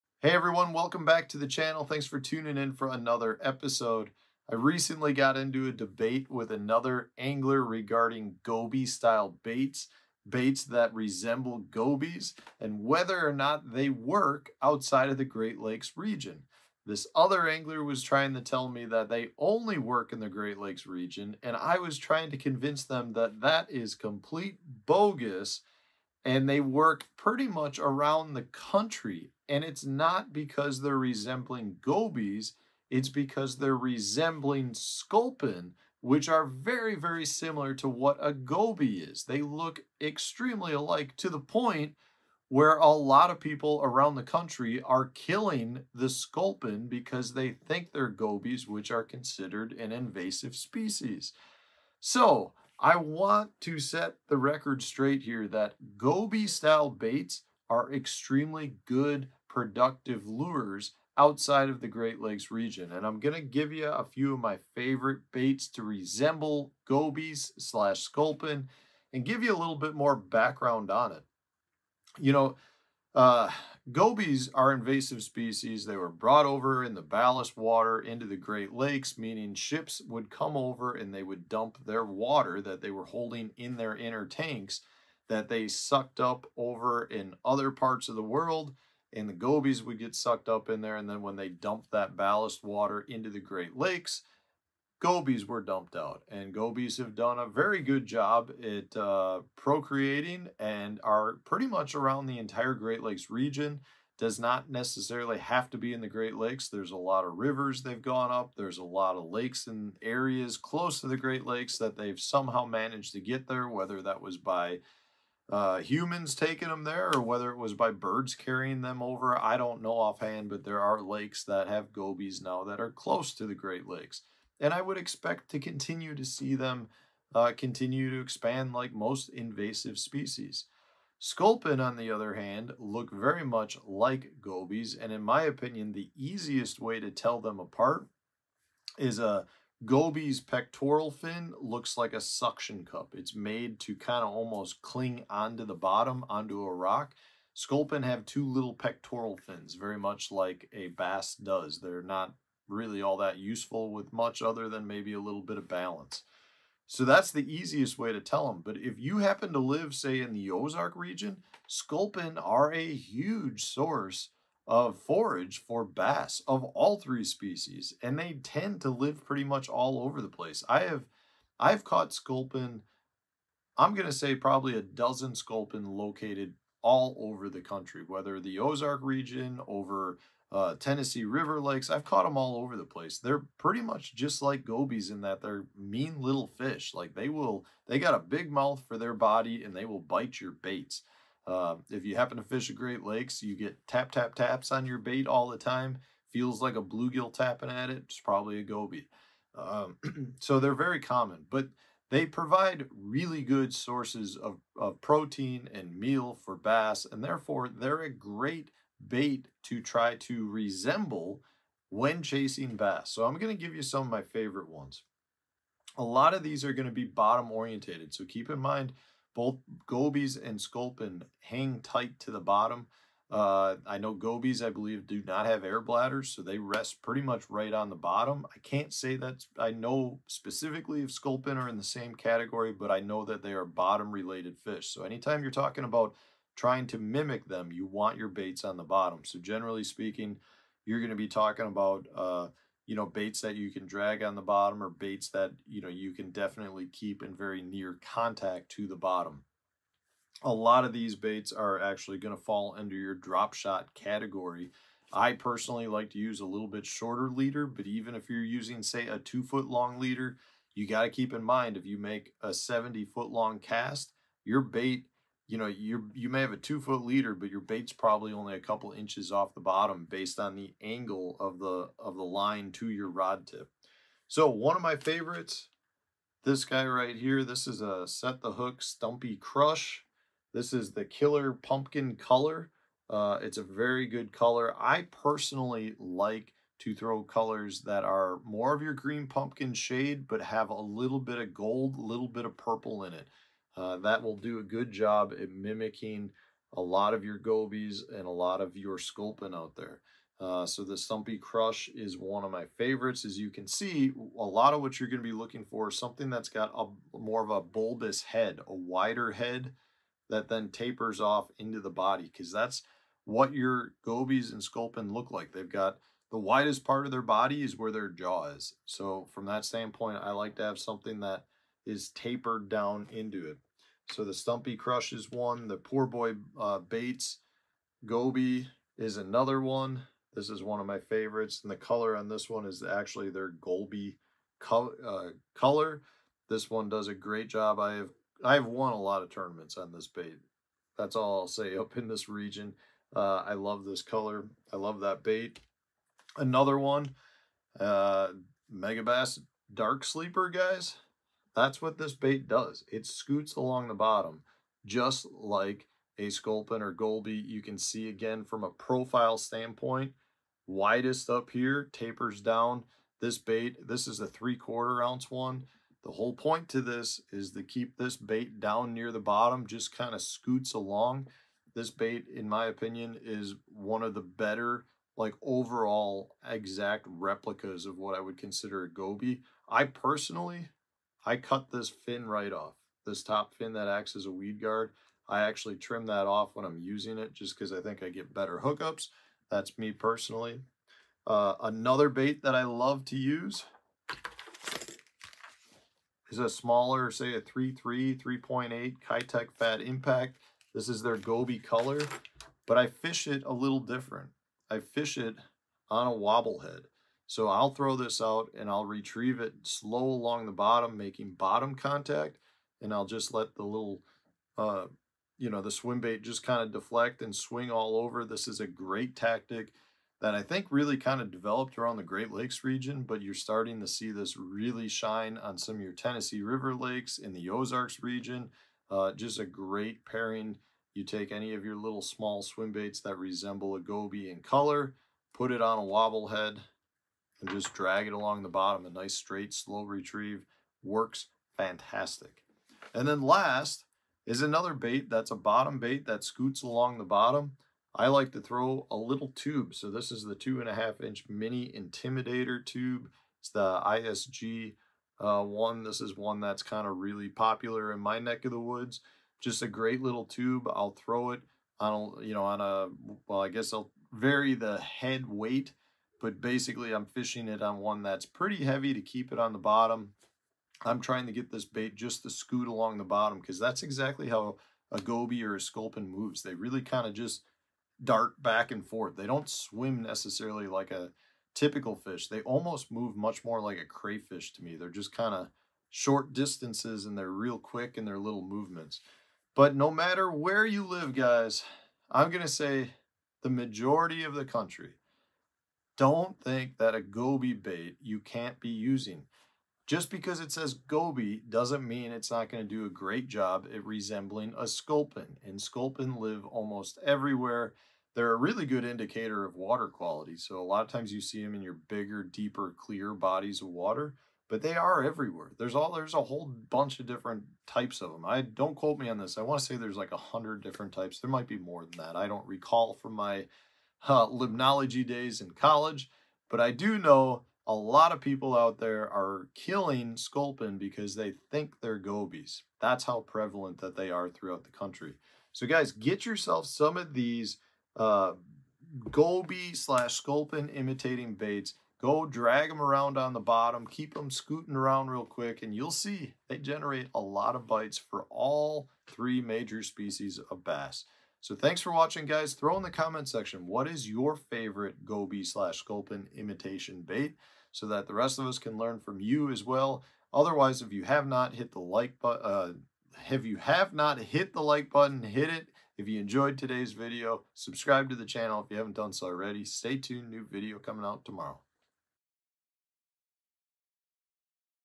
Do Goby Style Baits Work Everywhere?
Most people assume goby baits which are common around the Great Lakes only work in that region, but here’s why they work everywhere.

2) Share my videos on your social media platforms!
4) If you enjoy the content and want to see more of it.  Consider donating to the channel to show your support for future video creation.  Very much appreciated!